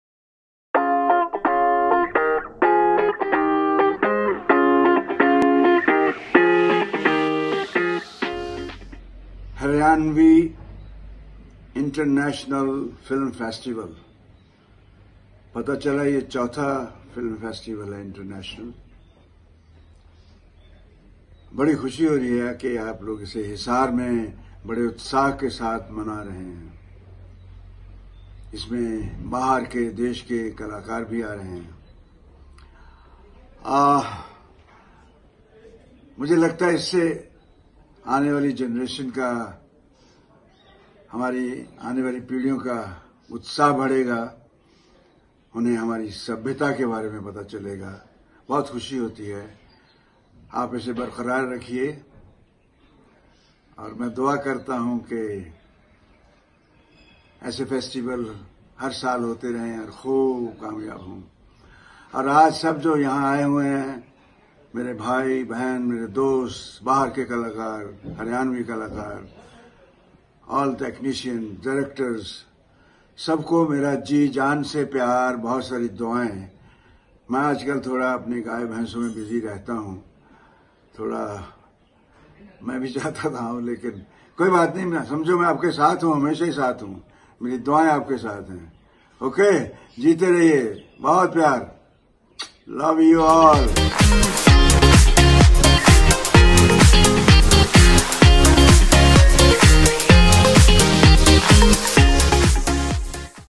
HE-MAN of Bollywood, Living Legend Dharmendra Ji promoting Haryana International Film Festival
4th Haryana International Film Festival